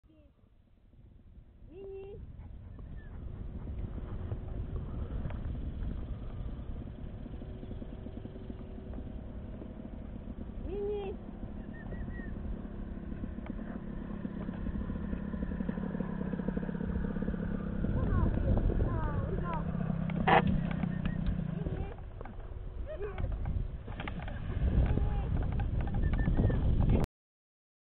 Mac landing
Macs landing after his first flight of 2007 after about a year of no flights!